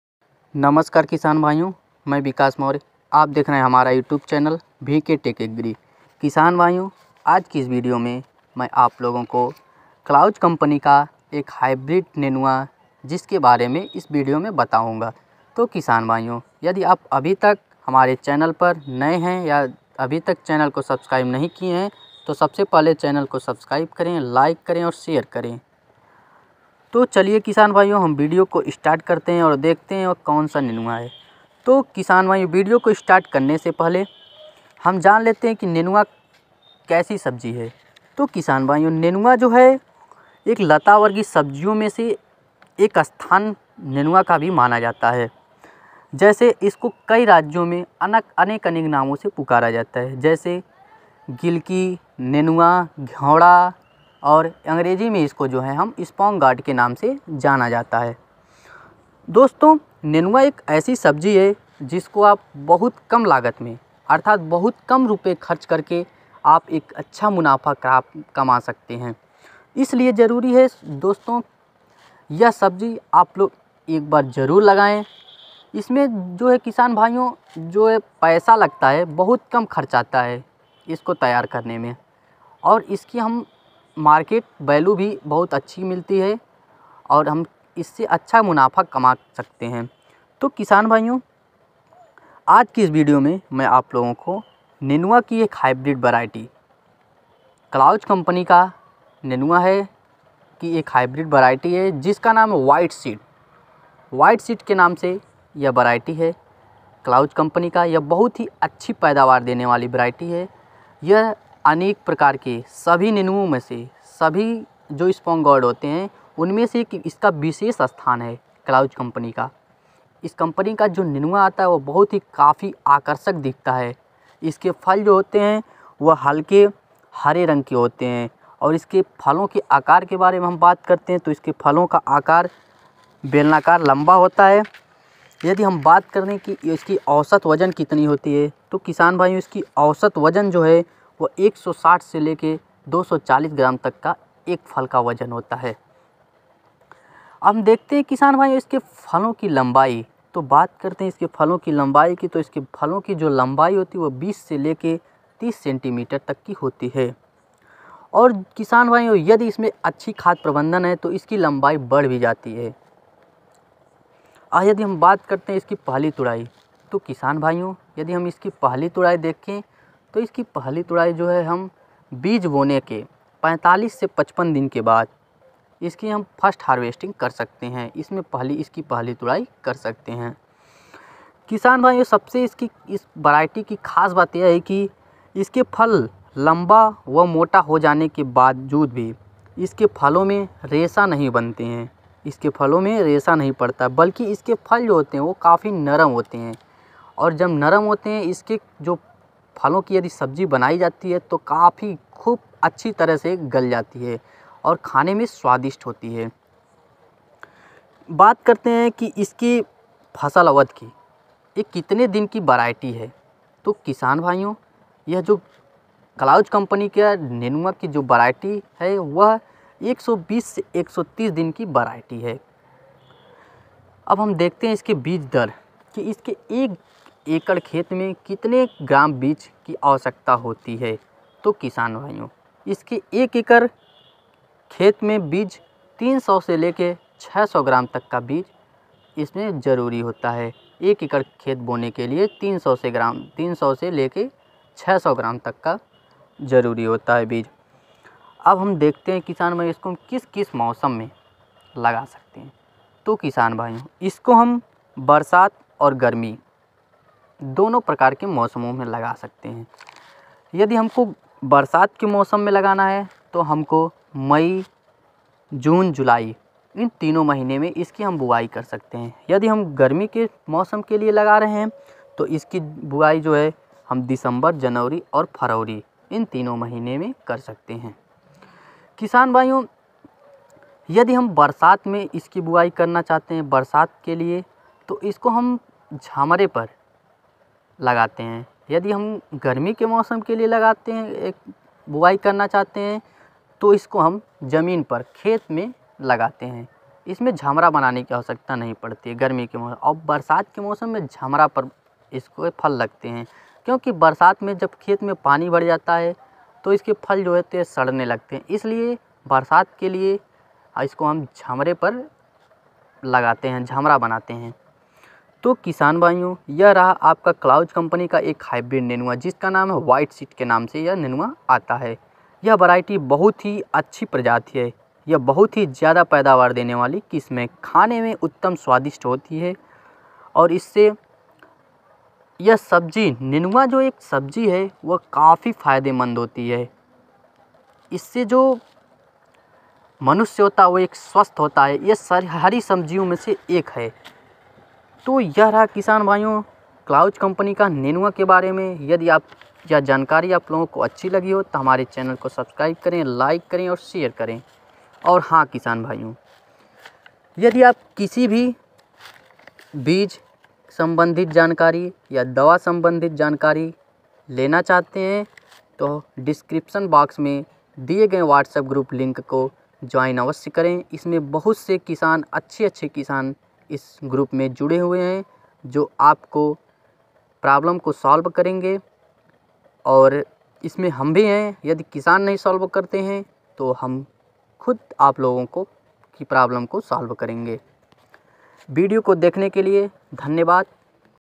Clause Sponge Guard White Seed Hybrid nenua beej . नेनुआ बीज । नेनुआ घेवड़ा spnogeguard.
नमस्कार,
     किसान भाईयों।

आज की इस Video में मैं आप लोगों को नेनुआ की एक हाइब्रिड variety के बारे में बताऊंगा।

किसान भाईयों यदि आप लोगों को किसी भी प्रकार के बीज संबंधित जानकारी और दवा के बारे में कोई जानकारी जानने के लिए,,,,,,
            नीचे दिए गए लिंक को join जरूर करें।

Thanks 💖